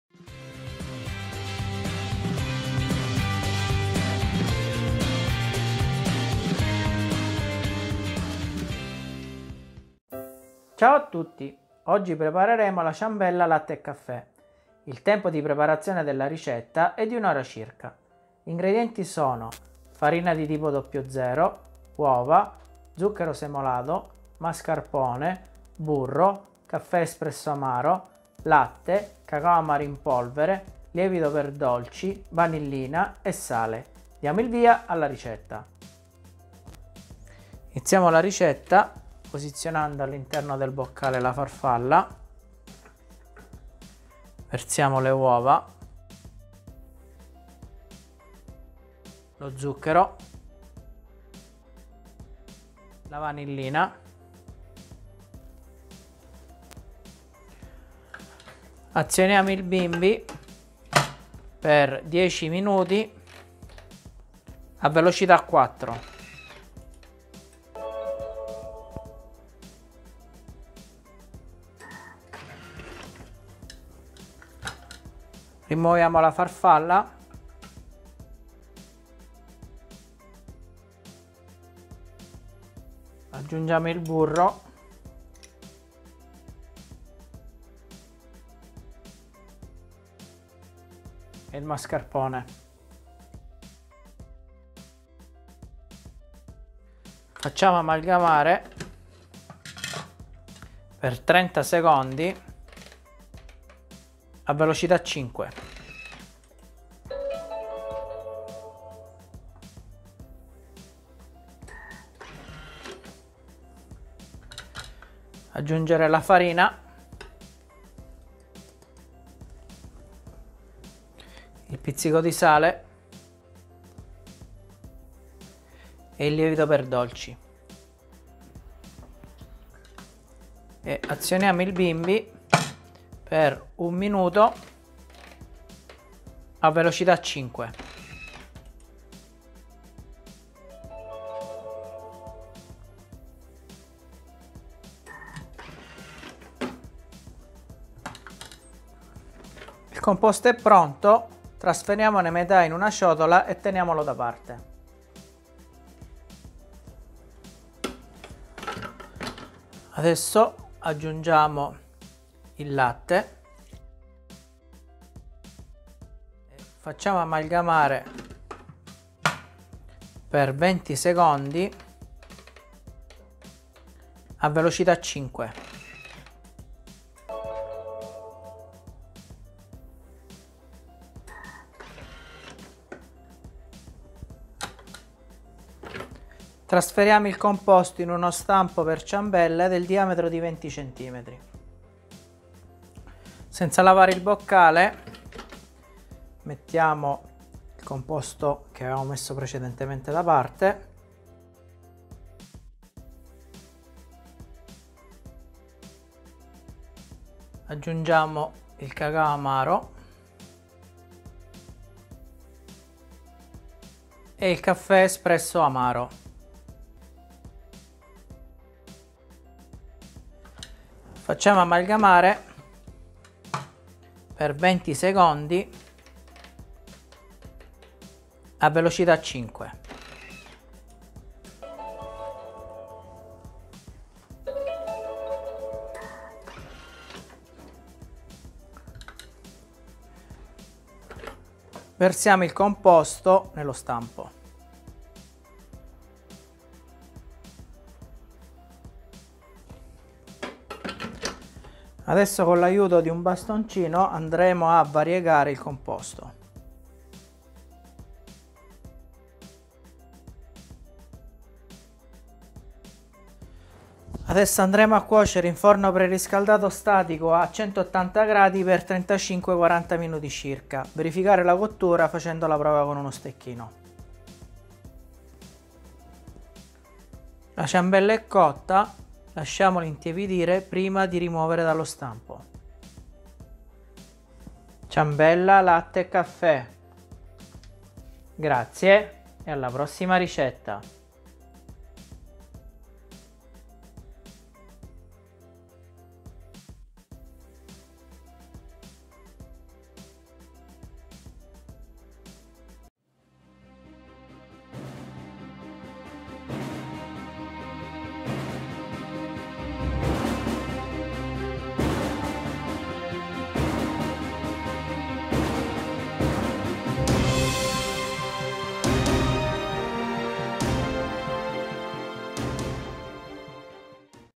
Ciambella latte e caffè per bimby TM6 TM5 TM31
Ingredienti:
210 g di farina di tipo "00"
2 uova
150 g di zucchero semolato
200 g di mascarpone
50 g di burro fuso
40 g di caffè espresso ristretto amaro
40 g di latte parzialmente scremato
5 g cacao amaro
10 g di lievito per dolci vanigliato
1 bustina di vanillina
1 pizzico di sale